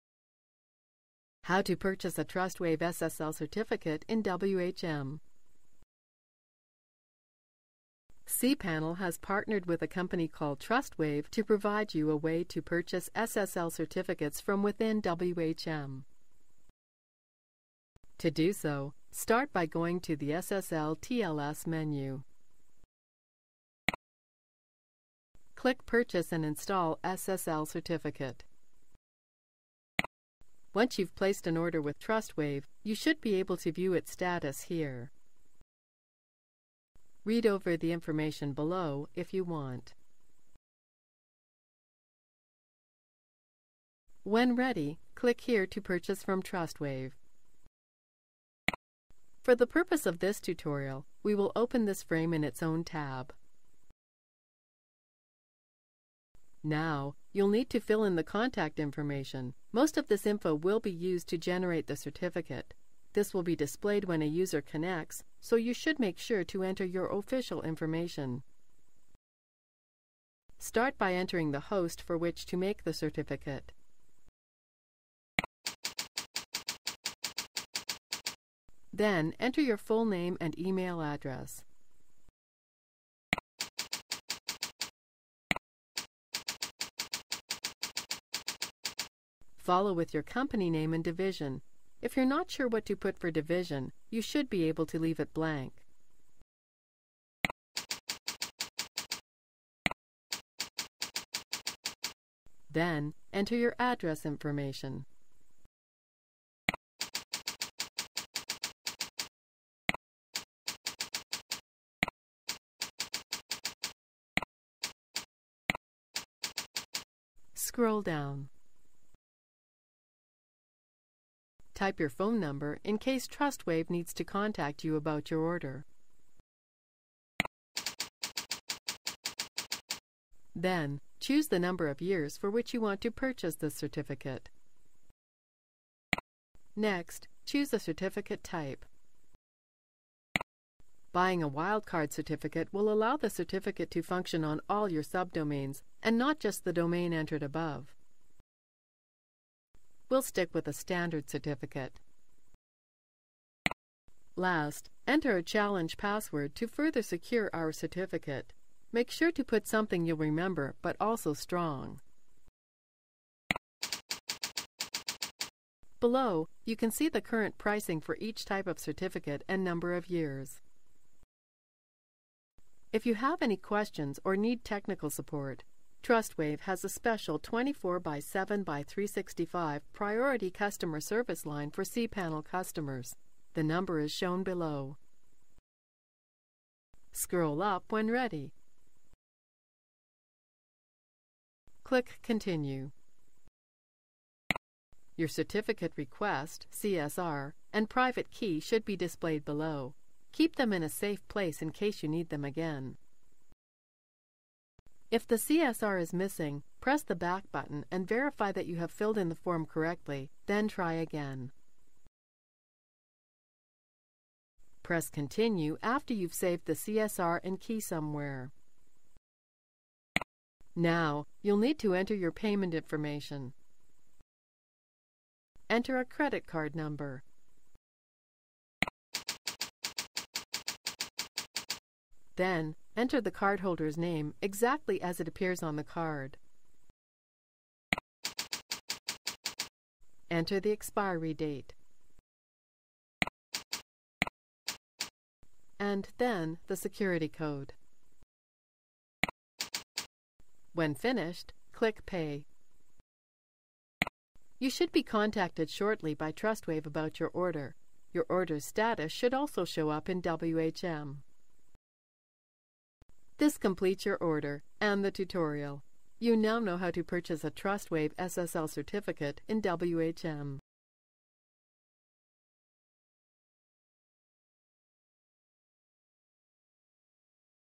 How to purchase a Trustwave SSL certificate in WHM - WHM Management Tutorials
How to purchase a Trustwave SSL certificate in WHM
• cPanel has partnered with Trustware to provide with a way to purchase the SSL certificates from within WHM.
• Go to SSL/TLS-Purchase & Install Certificate.
• After going through the written part, click on Click here to purchase from Trustwave.
• Enter all the details in the form and click Continue.
• Save the CSR and the key and click on Continue.
• Enter the payment details and when done, click on Pay.